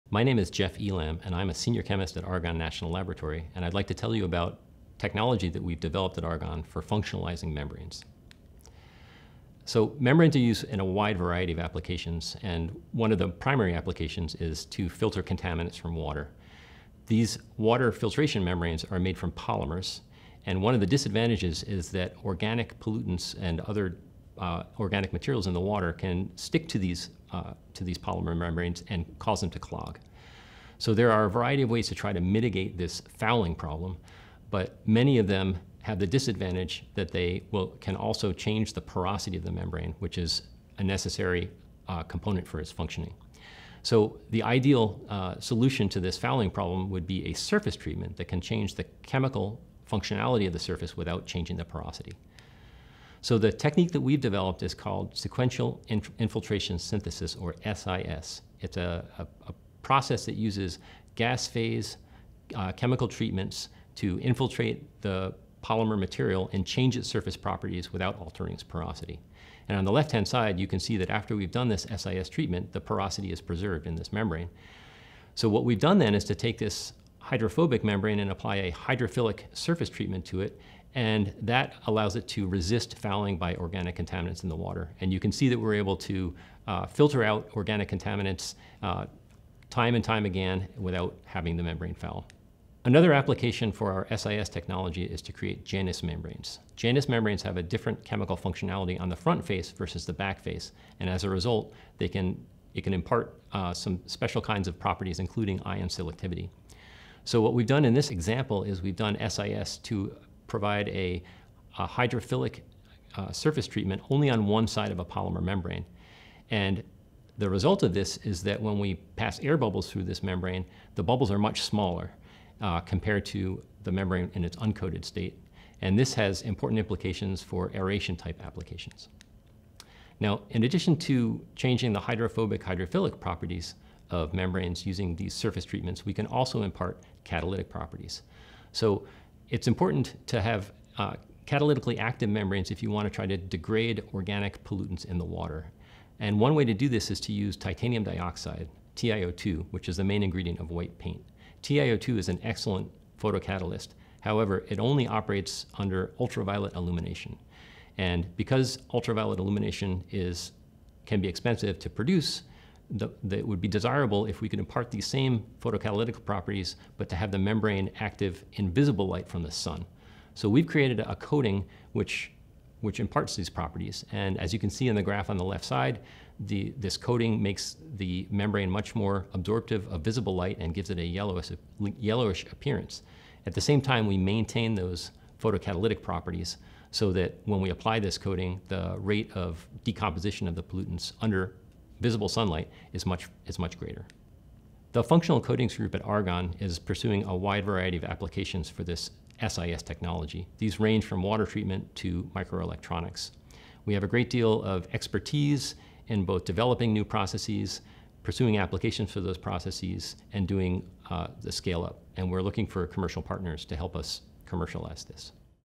Advanced Membrane Functionalization // Jeff Elam, Argonne
Jeff Elam, Argonne // Advanced Membrane Functionalization

The performance of membranes is determined in large part by the surface properties of the material, especially along the pore walls. Commercial membranes are manufactured from a relatively limited set of polymer materials, which provide a limited set of interfacial properties. Most of these polymers are hydrophobic, which makes them prone to fouling, especially in challenging separations such as oil/water. Fouling of membranes during industrial separations necessitates periodic cleaning with sparging or harsh chemicals (many of which degrade the structural integrity of the membrane), and ultimately replacement of the membranes. Most approaches to coating membranes to endow them with fouling resistance result in pore blockage due to non-conformal deposition.

Argonne uses conformal interface modification techniques such as atomic layer deposition (ALD) to modify the membrane surface properties without constricting the pores. Using these techniques, we can transform existing commercial polymer membranes into hydrophilic ones that passively resist the attachment of foulants, enabling substantially extended operational lifetime between cleanings as well as easier cleaning. For even more challenging fouling environments, ALD coatings can be rendered catalytically active so that reactive oxygen species can be formed at the pore walls in operando, degrading and detaching any foulants that may adhere.

ALD also has the ability to create gradient coatings along the pore lengths, producing so-called Janus membranes with different properties on their two sides. These membranes can exhibit transport properties impossible to achieve using traditional membranes, such as unidirectional transport of ions.

// This technology was featured in the License to Innovate event – a showcase of select IP and research capabilities at the University of Chicago, Argonne National Laboratory, and Fermi National Accelerator Laboratory.